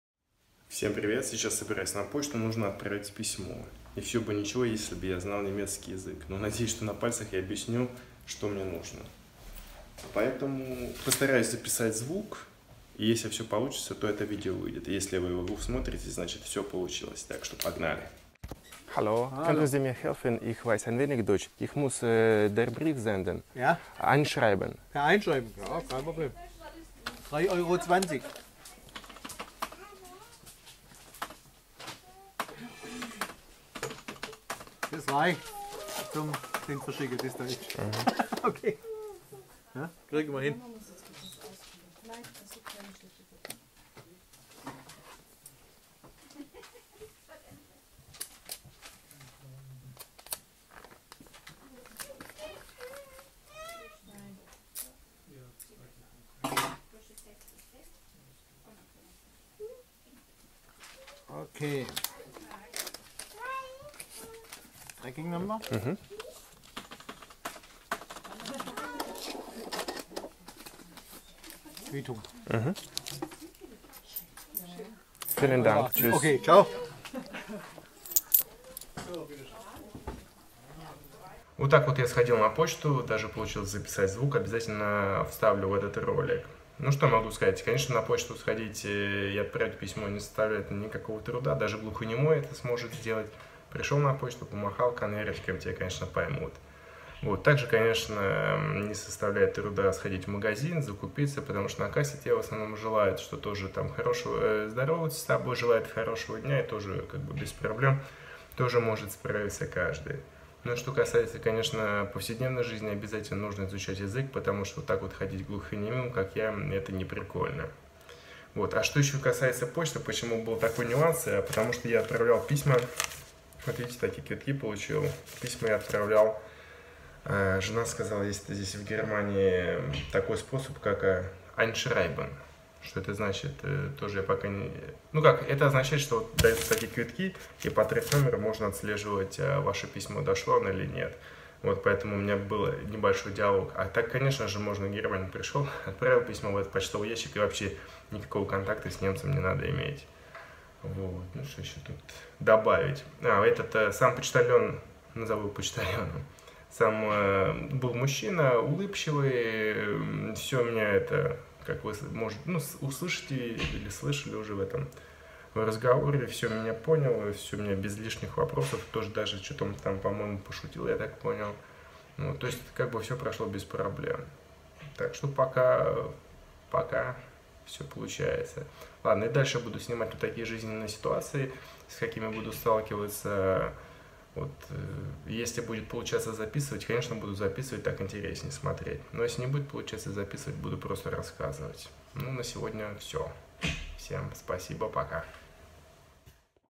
Как я ходил на Deutsche Post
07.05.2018
Ходил самостоятельно на почту, отправлял письмо.